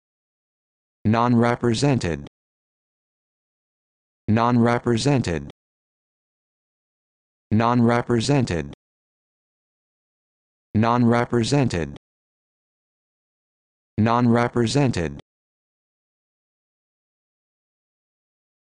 How to Pronounce Nonrepresented
This video shows you how to pronounce Nonrepresented